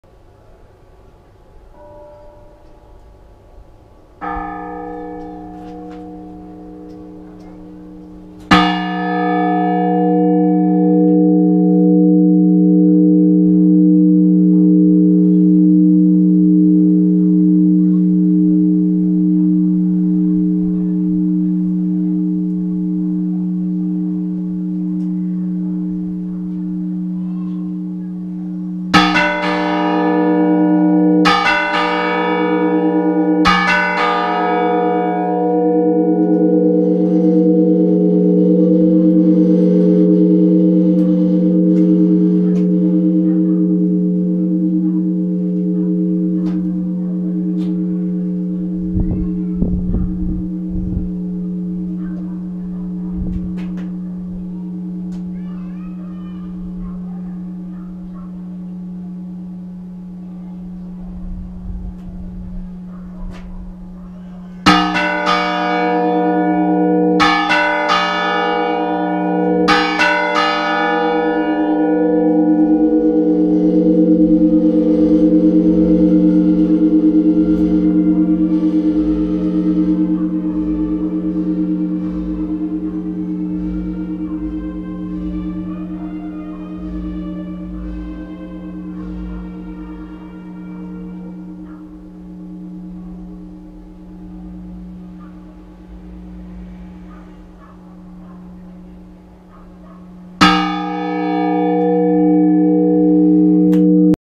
Toque de Consagracion de Escalonilla.
El toque de consagración es un repique de campanas que marca el momento de la consagración del pan y e vino en Cuerpo y Sangre de Cristo.
Antiguamente las campanas también informaban sobre el transcurso de la misa, tocando en el Gloria y en la Consagración. Este es uno de ellos.

Consiste en una campanada de la grande en el momento de comenzar la consagracion.
En el momento de alzar la Ostia (el pan) Se repite tres veces tres campanadas: (1+2+1) (1+2+1) (1+2+1) (Siendo 1 la campana grande y 2 la campana pequeña)
En el momento de Alzar el Cáliz se hace lo mismo que con el Pan y acabar la consagración se da una campanada de la grande igual que al principio.